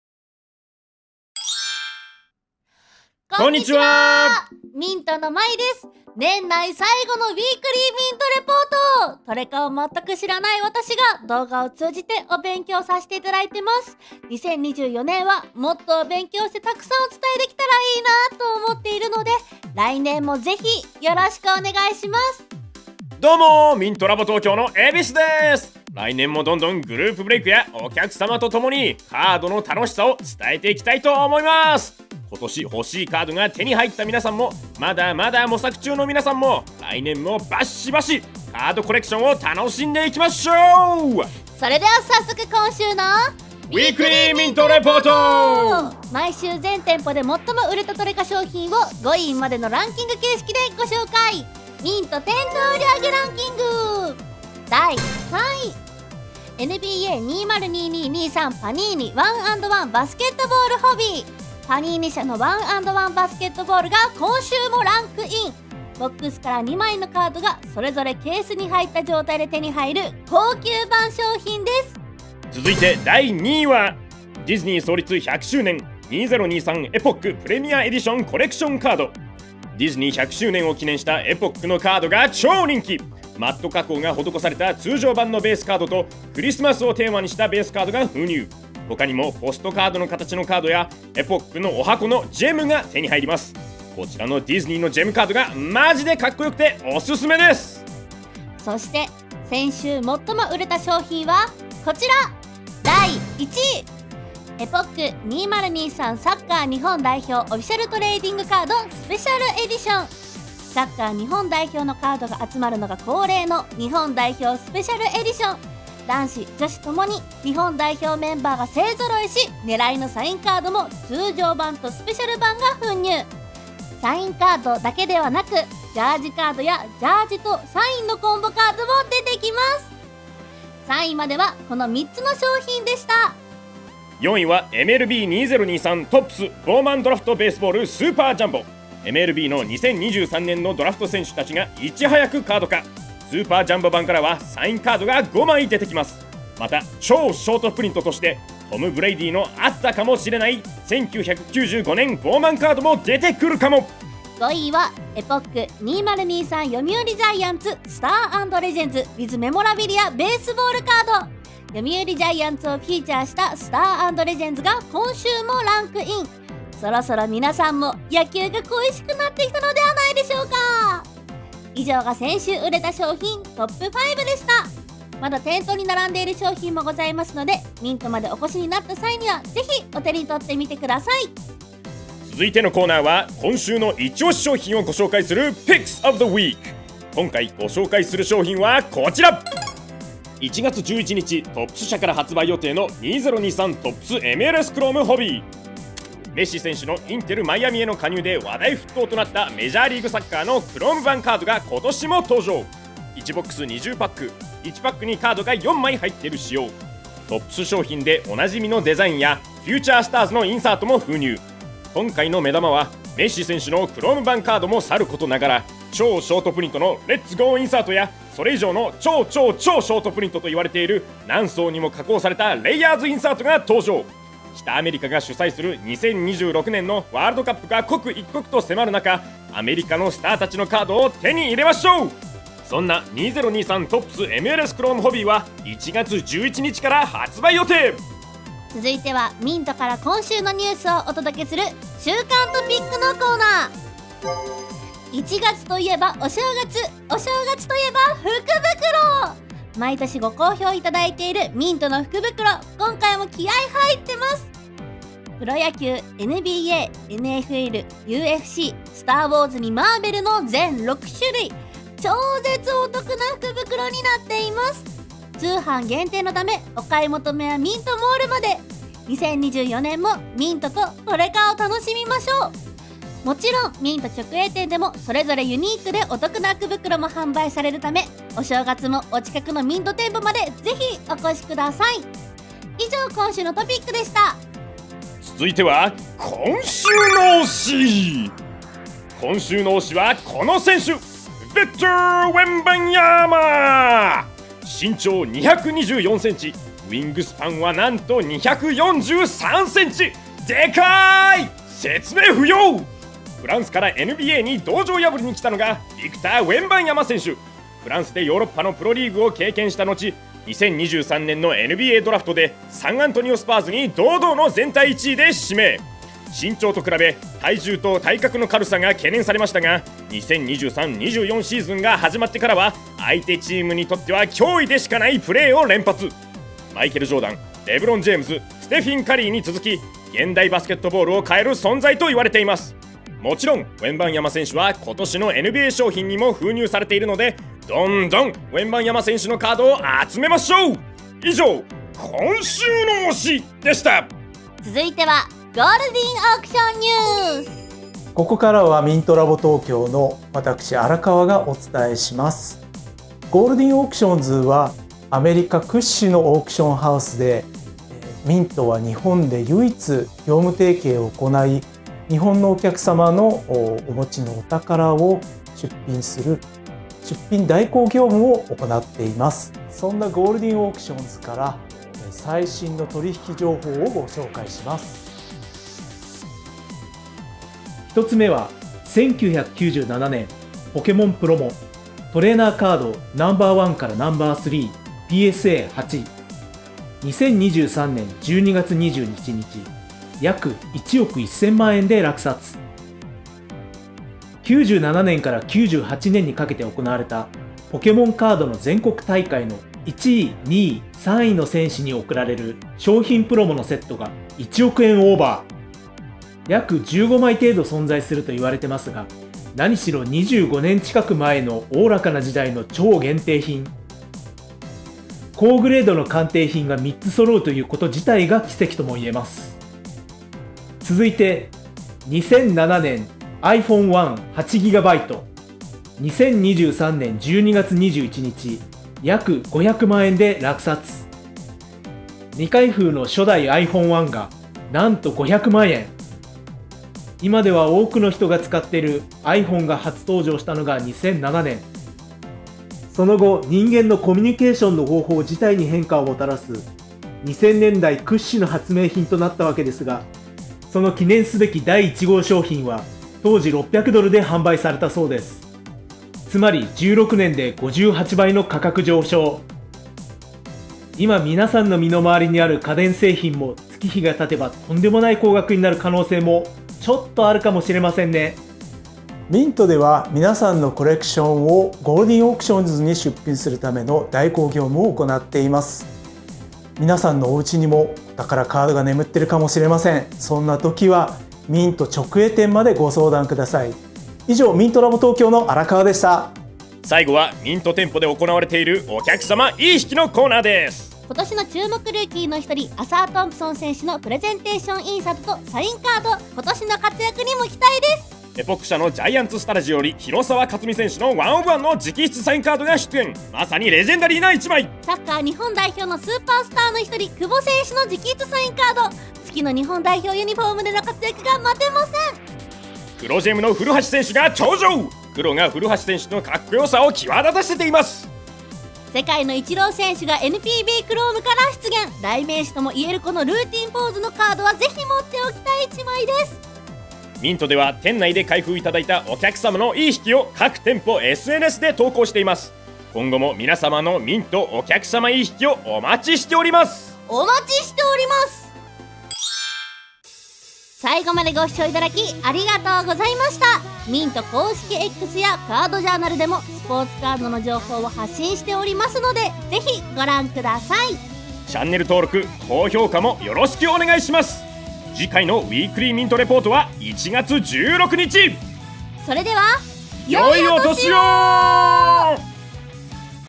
【超高額取引】ポケカのPSA8が1億円越え！？｜Weekly MINT Report #17
○MINT Web